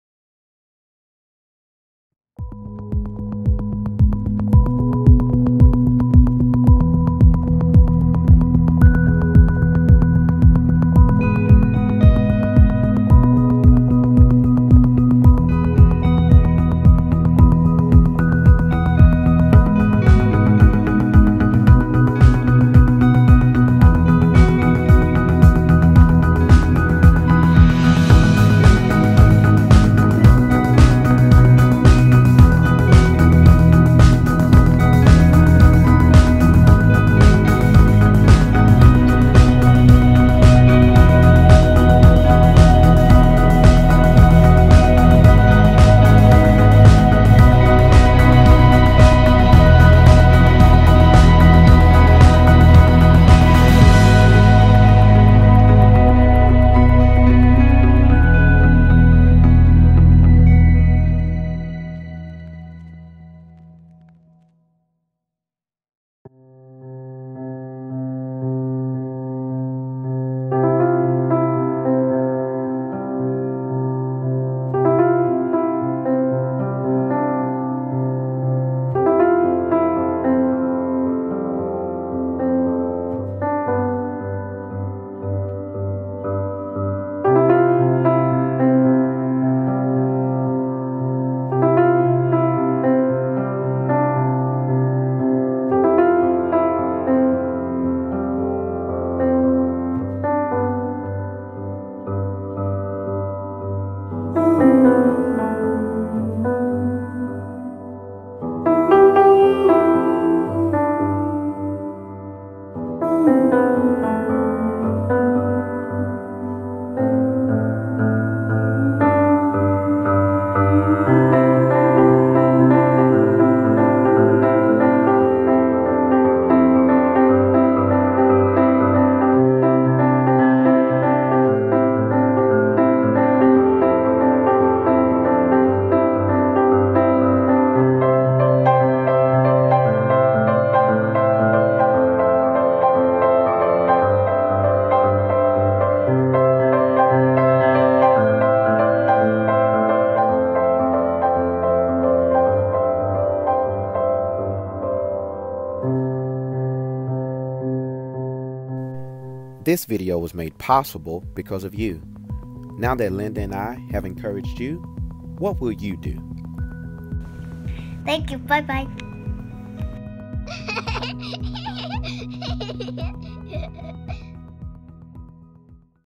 Special Tribute for "You" 3yr old encouraging dad that he is doing a good job..
We created this video as a token of our appreciation for over 6 million views on multiple Social Media Websites in 10 days... New York Times, MSNBC, Cosmopolitan, WorldstarHiphop, TVONE, Disney Channel and so many more... Why Did the video go viral you might ask??? It's touching hearts all over the world in just 87 seconds.. The Culpepper's are committed to making a positive change by warming hearts, bringing smiles and encouraging people in the Lord everywhere we go. Continue to share the Love. Feel free to follow the Culpepper's on FB